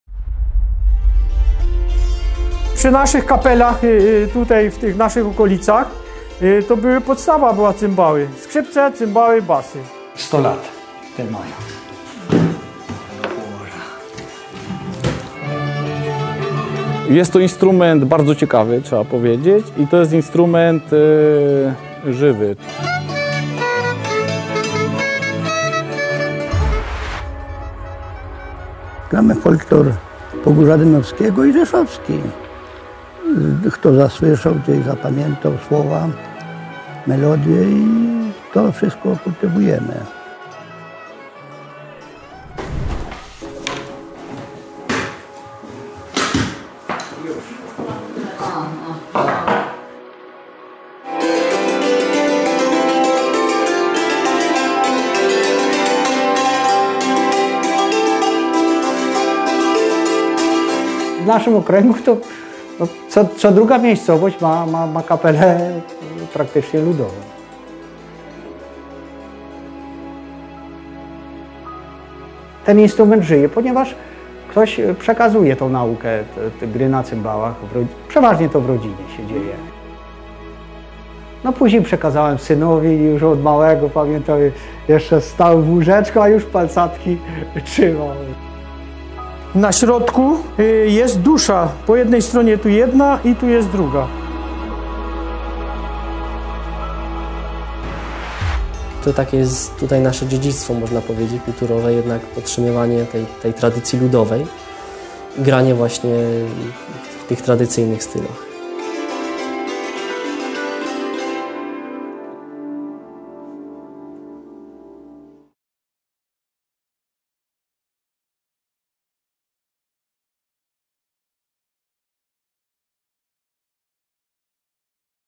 Zapowiedź - 39. Spotkania Cymbalistów
W dniach 11-13 grudnia 2020 r. Wojewódzki Dom Kultury w Rzeszowie organizuje 39. Spotkania Cymbalistów. Wydarzenie odbędzie się on-line i będzie transmitowane na żywo.

Coroczne „Spotkania Cymbalistów” to unikatowy konkurs organizowany przez Wojewódzki Dom Kultury w Rzeszowie już 39 raz. Konkurs ma niepowtarzalny w skali kraju charakter. Ocala od zapomnienia sztukę gry na cymbałach – tradycyjnym, charakterystycznym dla Podkarpacia instrumencie. Niezwykle istotne jest stwarzanie ludowym muzykom możliwości do zaprezentowania sztuki gry oraz integracja i wymiana doświadczeń uczestników konkursu. W tym roku ze względu na zaistniałą sytuację całość Spotkań odbędzie się on-line i będzie transmitowana na żywo.

39. Spotkania Cymbalistów zostały dofinansowane przez Ministra Kultury i Dziedzictwa Narodowego ze środków pochodzących z Funduszu Promocji Kultury. Finansowo Konkurs wsparł oraz objął Honorowym Patronatem Marszałek Województwa Podkarpackiego.